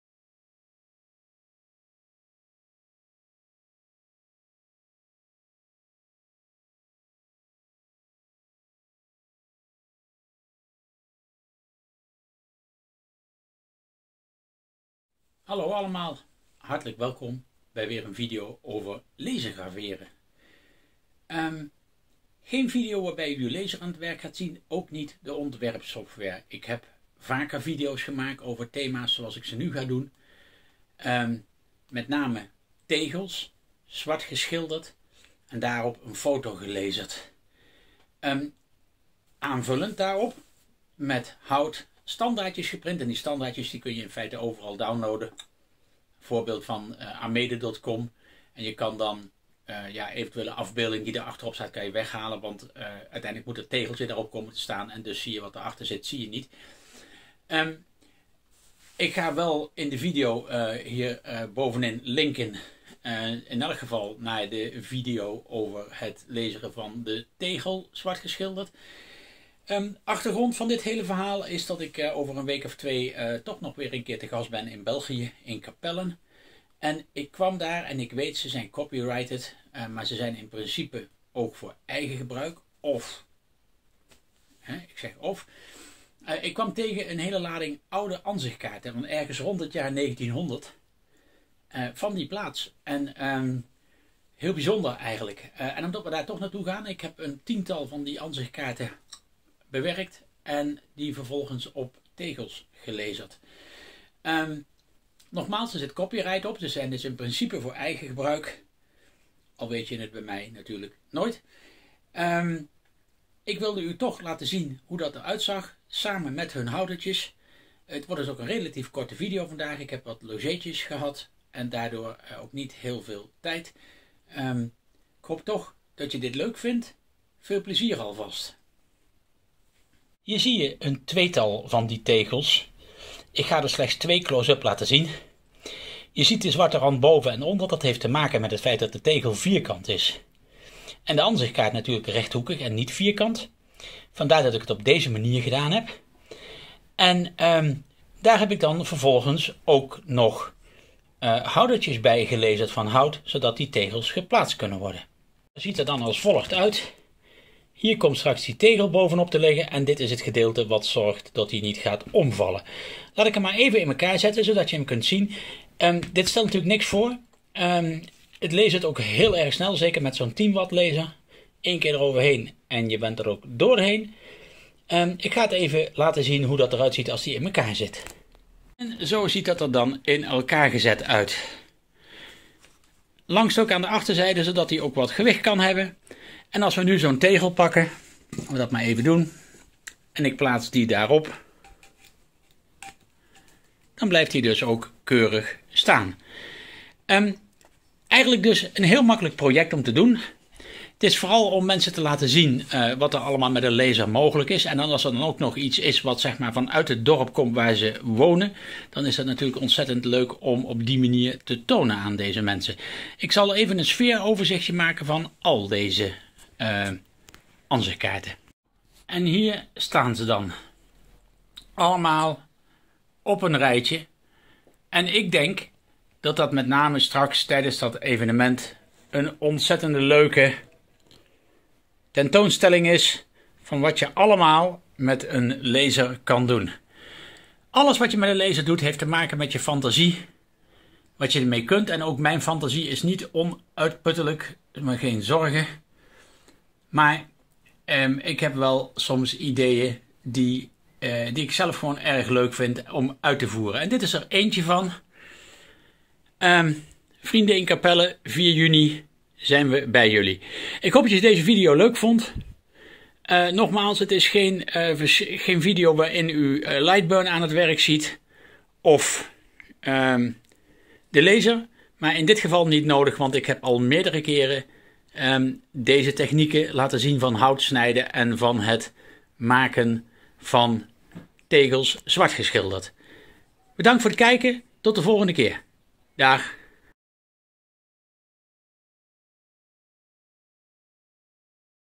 Ortur Laser Master 3 Ansichtkaarten op tegels
Dit keer een korte video met weer een idee van wat je kunt laseren en misschien zelfs wel verkopen.